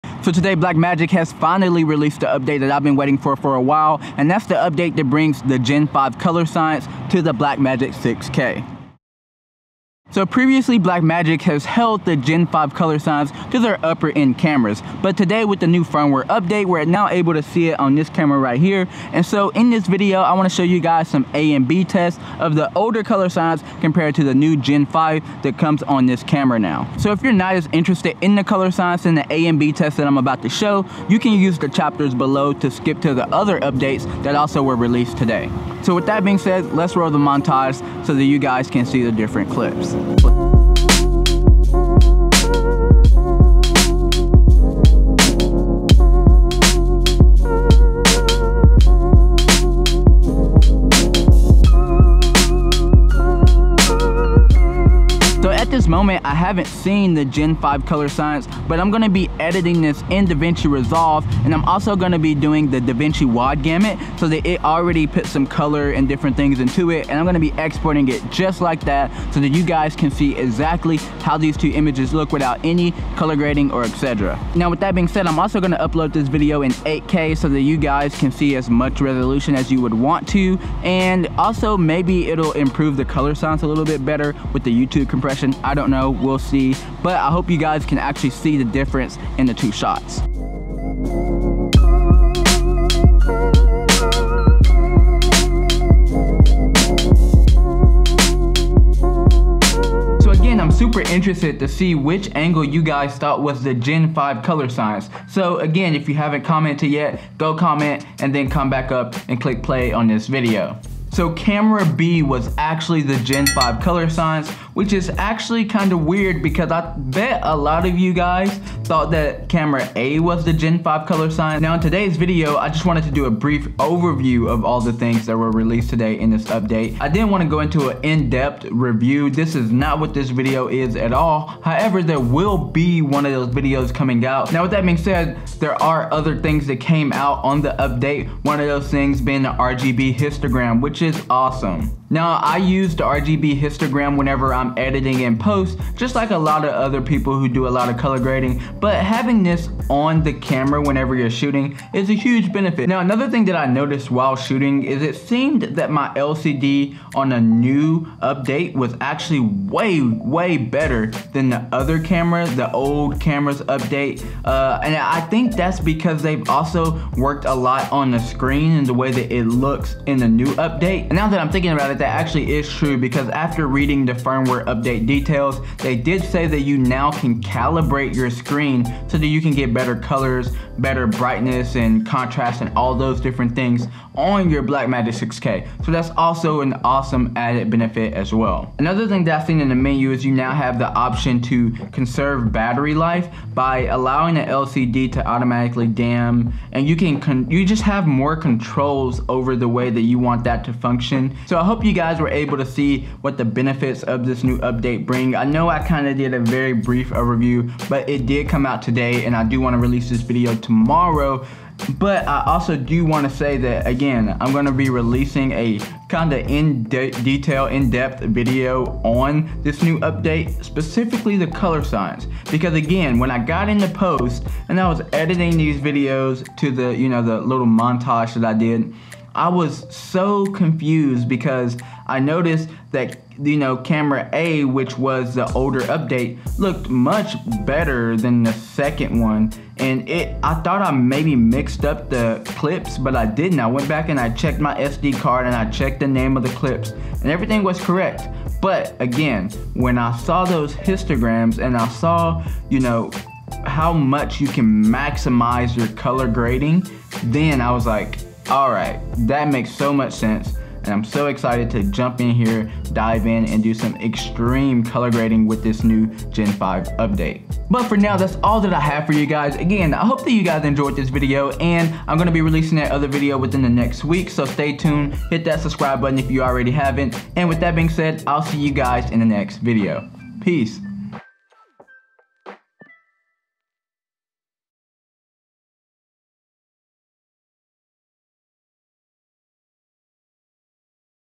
Blackmagic 6K Camera Update 7.3 | RGB Histogram | Gen 5 Color Science & More
Today I wanted to do a brief overview and A/B test of the raw files of the new Blackmagic 6k pro and bmpcc6k update! Excited for all the new updates, RGB Histogram, Gen 5 Color Science, Screen Calibration, and more! Davinci Resolve 17 is a powerful program and I'm happy to grade this footage in it.